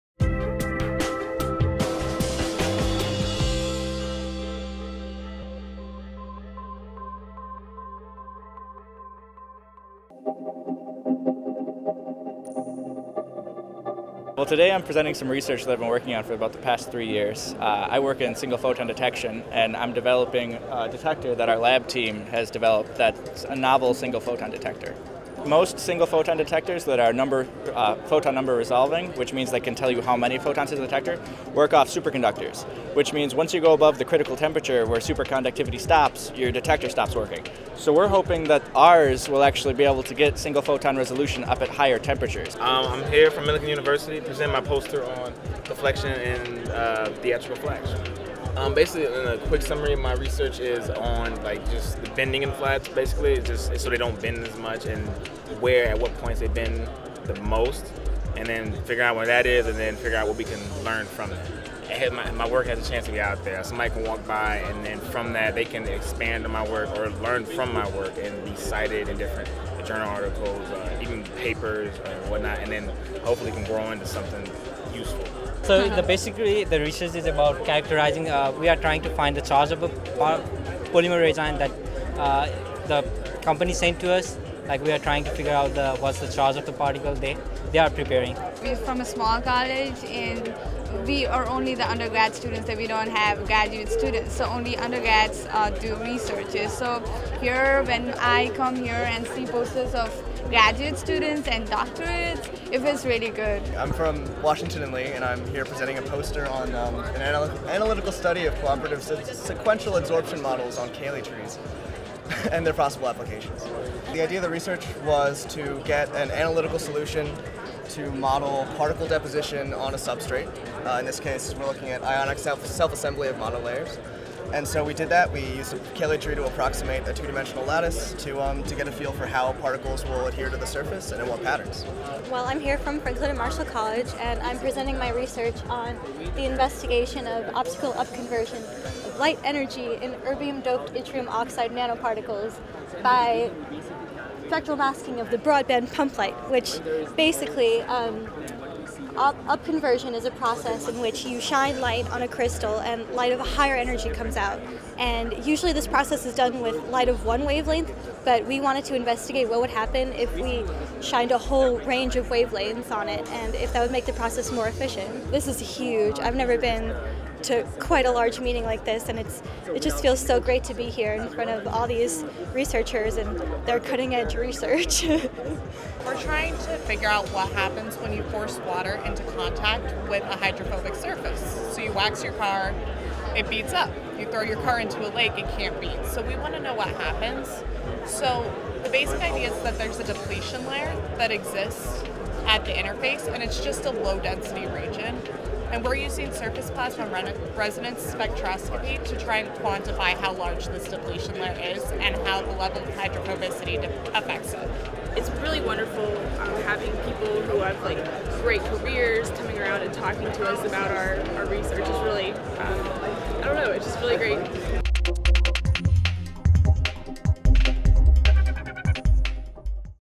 Student Poster Sessions at APS March Meeting 2013
Undergrads present their research at the APS March Meeting 2013 in Baltimore.